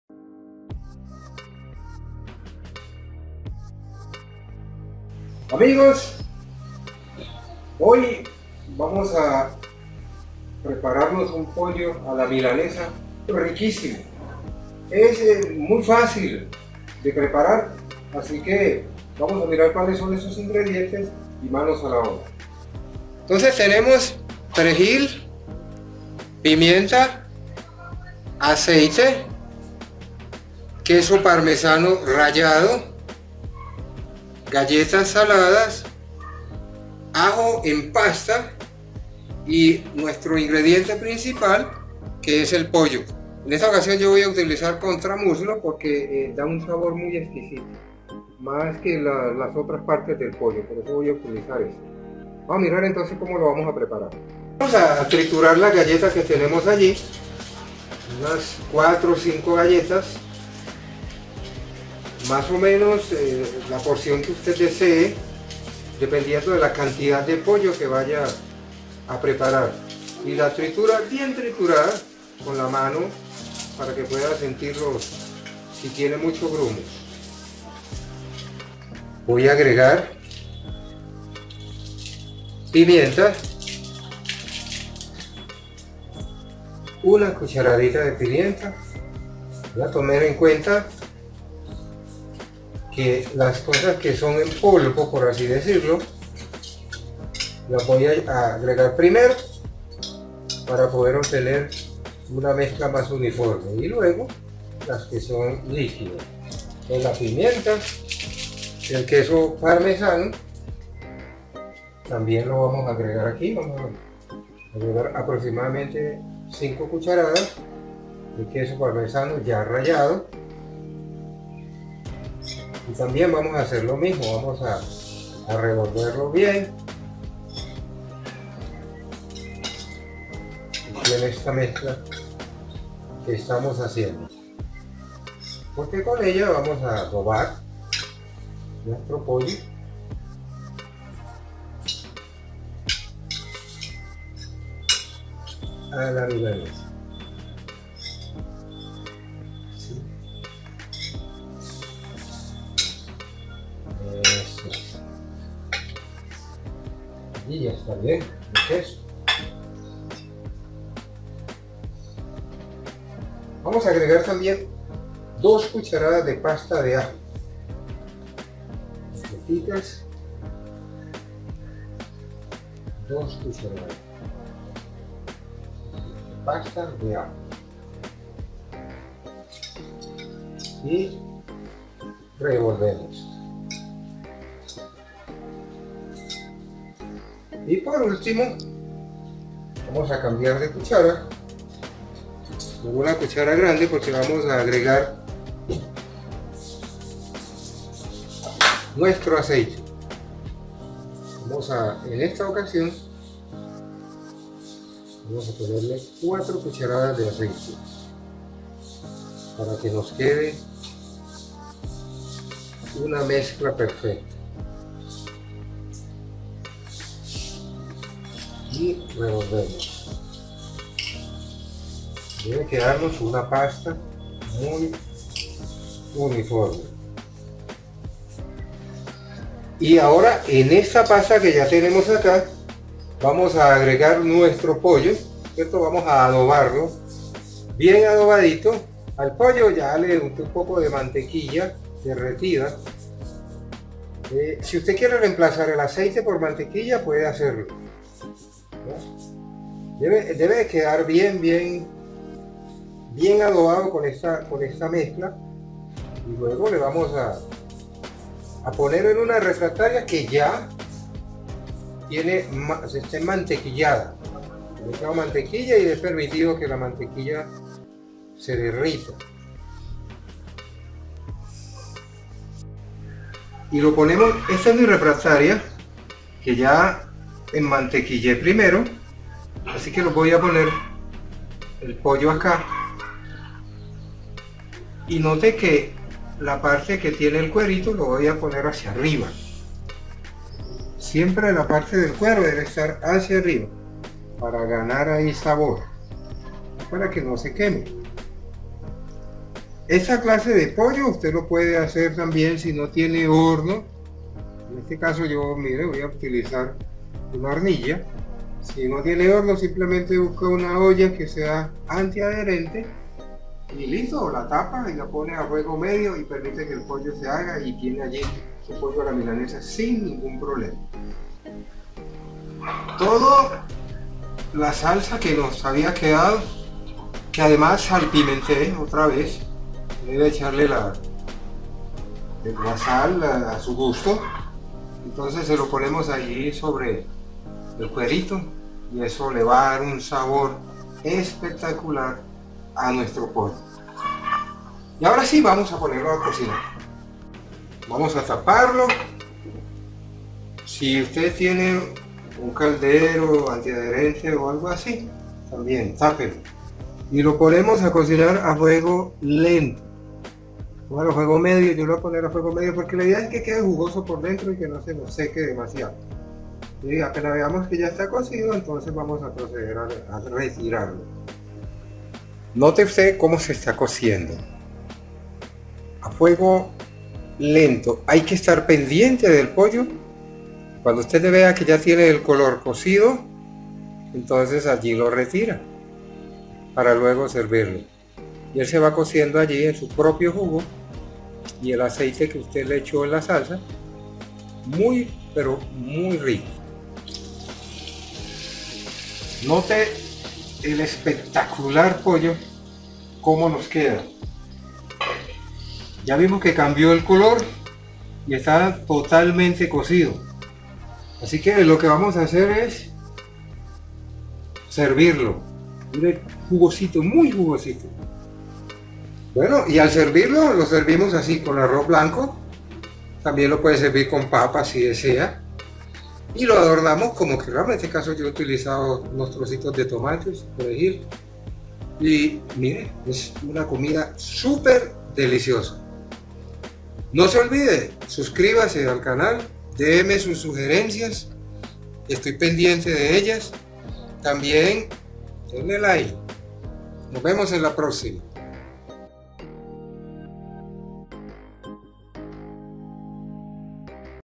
Receta de Milanasa de Pollo
Una preparación un poco diferente del pollo a la milanesa, es un pollo muy rico que permite muchas presentaciones y el pollo a la milanesa es fácil de hacer y requiere de poco tiempo